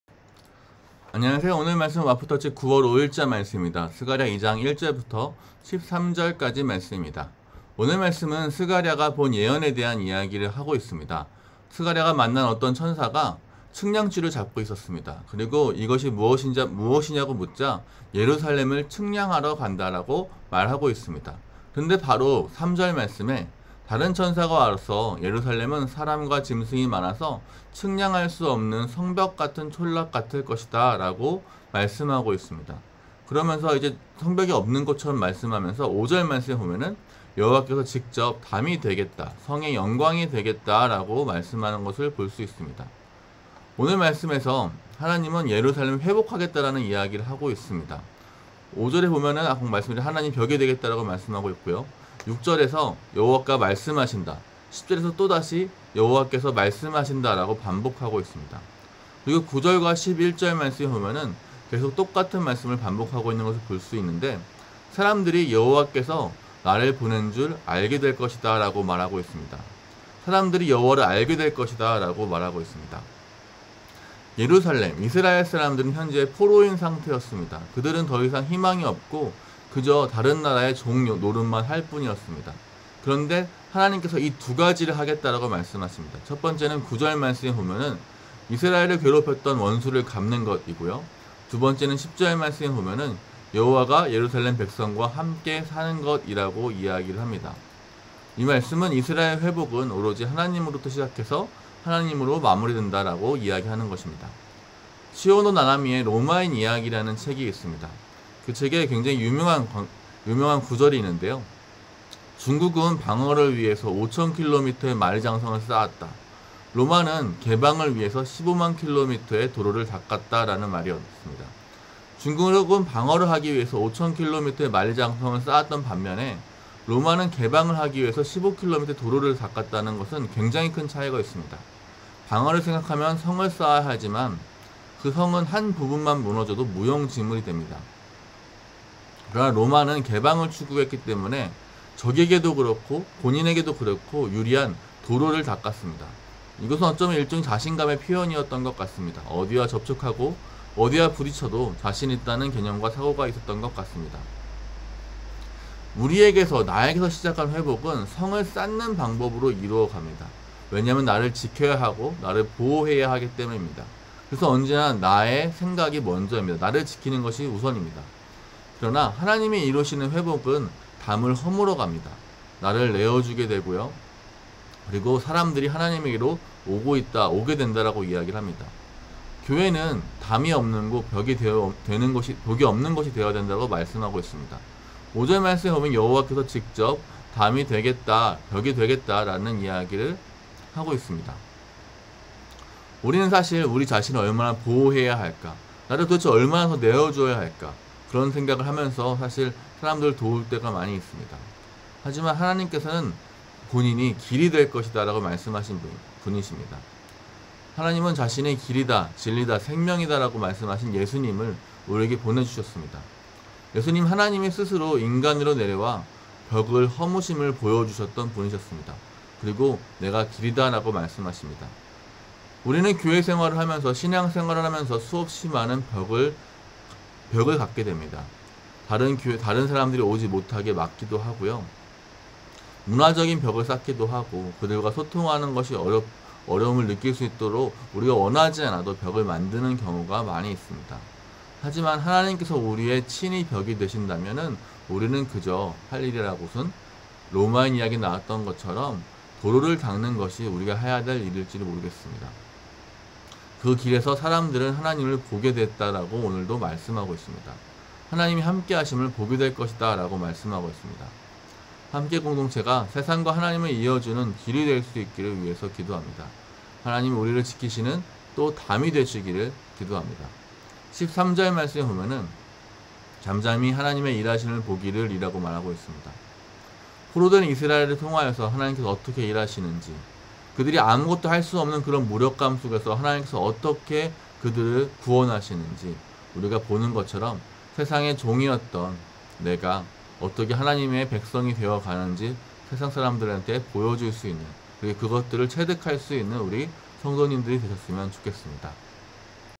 [아침N묵상] 9월 5일(화) | 와플터치QT | 스가랴 2:1-13 (정헌택)
1내가 또 눈을 들어 본즉 한 사람이 측량줄을 그의 손에 잡았기로

2네가 어디로 가느냐 물은즉 그가 내게 대답하되 예루살렘을 측량하여 그 너비와 길이를 보고자 하노라 하고 말할 때에

3내게 말하는 천사가 나가고 다른 천사가 나와서 그를 맞으며

4이르되 너는 달려가서 그 소년에게 말하여 이르기를 예루살렘은 그 가운데 사람과 가축이 많으므로 성곽 없는 성읍이 될 것이라 하라

5여호와의 말씀에 내가 불로 둘러싼 성곽이 되며 그 가운데에서 영광이 되리라

6오호라 너희는 북방 땅에서 도피할지어다 여호와의 말씀이니라 이는 내가 너희를 하늘 사방에 바람 같이 흩어지게 하였음이니라 여호와의 말씀이니라

7바벨론 성에 거주하는 시온아 이제 너는 피할지니라

8만군의 여호와께서 이같이 말씀하시되 영광을 위하여 나를 너희를 노략한 여러 나라로 보내셨나니 너희를 범하는 자는 그의 눈동자를 범하는 것이라

9내가 손을 그들 위에 움직인즉 그들이 자기를 섬기던 자들에게 노략거리가 되리라 하셨나니 너희가 만군의 여호와께서 나를 보내신 줄 알리라

10여호와의 말씀에 시온의 딸아 노래하고 기뻐하라 이는 내가 와서 네 가운데에 머물 것임이라

11그 날에 많은 나라가 여호와께 속하여 내 백성이 될 것이요 나는 네 가운데에 머물리라 네가 만군의 여호와께서 나를 네게 보내신 줄 알리라

12여호와께서 장차 유다를 거룩한 땅에서 자기 소유를 삼으시고 다시 예루살렘을 택하시리니

13모든 육체가 여호와 앞에서 잠잠할 것은 여호와께서 그의 거룩한 처소에서 일어나심이니라 하라 하더라

스가랴 2:1-13